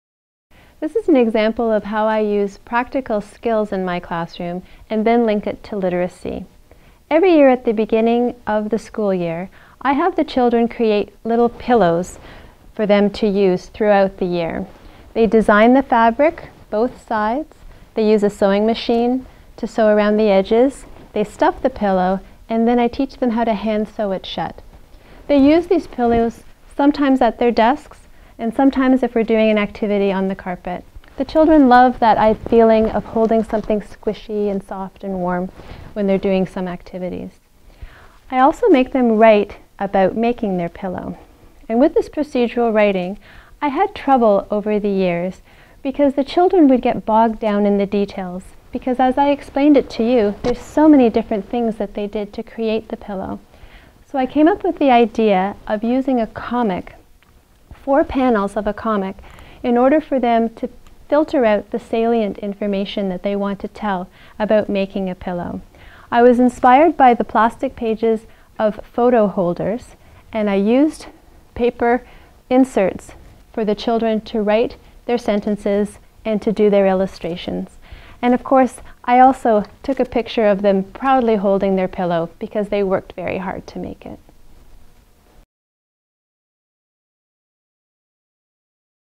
Pillow Making: Practicing Procedural Writing (Virtual Tour)
Writing about each step involved in making a pillow helps students develop procedural writing. and do a virtual tour of this teacher’s classroom for more free teaching activities and virtual classroom tours.